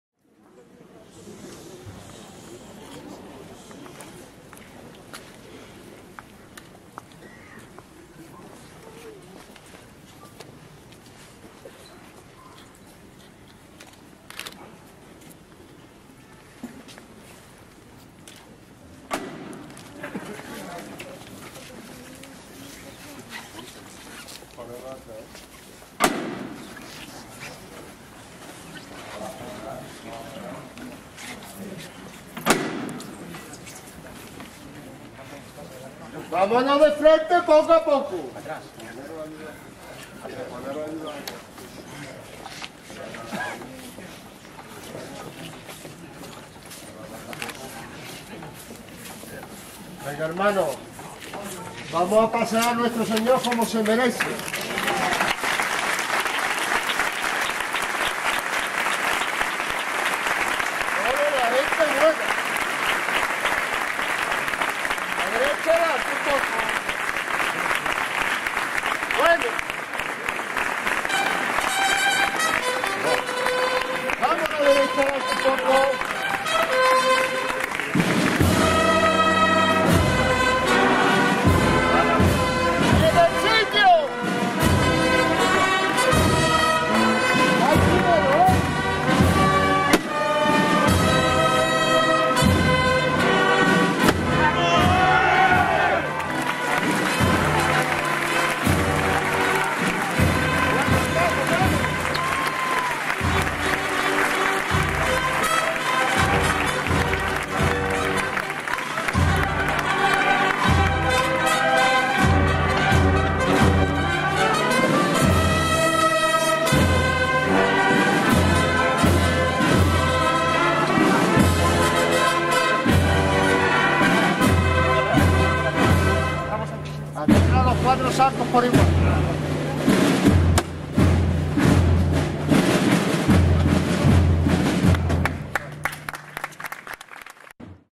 Salida Sentencia 2012
Madrugada de Viernes Santo, las puertas del edificio histórico de la Universidad de Oviedo se abren para que salga, en primer lugar el cortejo de nazarenos, y a continuación el paso de misterio de Nuestro Padre Jesús de la Sentencia. Carlos Milla, su capataz, dirige los pasos de los costaleros que salen de rodillas.